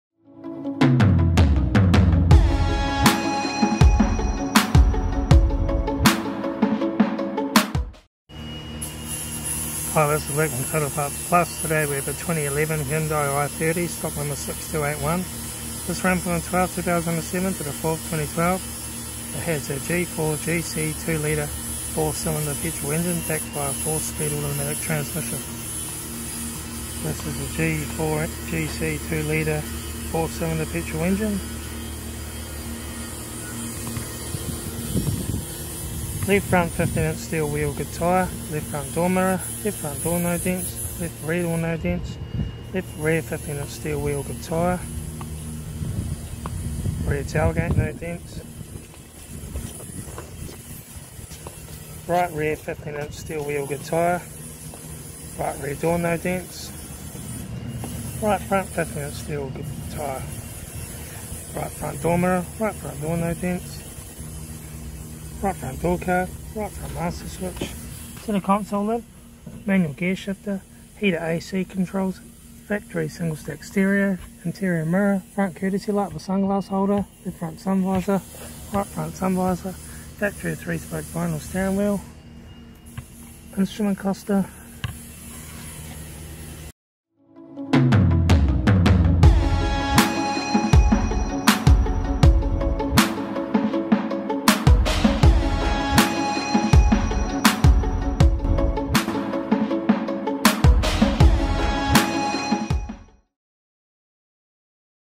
Wrecking – 2011 Hyundai i30 – Manual FWD 6281
2011 Hyundai i30 – Manual 6281
2.0 Litre 4 Cylinder Petrol
Stock # 6281
Hi, this is Luke from Total Parts Plus. Today we have 2011 Hyundai i30, stock number 6281.
This ran from the 12th, 2007 to the 4th, 2012. It has a G4GC 2.0 litre four-cylinder petrol engine, backed by a four-speed automatic transmission. The is the G4GC 2.0 litre four-cylinder petrol engine.
Right front door guard. Right front master switch. Centre console lid. Manual gear shifter. Heater, AC controls. Factory single-stack stereo. Interior mirror. Front courtesy light with sunglass holder. Left front sun visor. Right front sun visor. Factory three-spoke vinyl steering wheel. Instrument cluster.

Ebay Store:  Total Parts Plus 1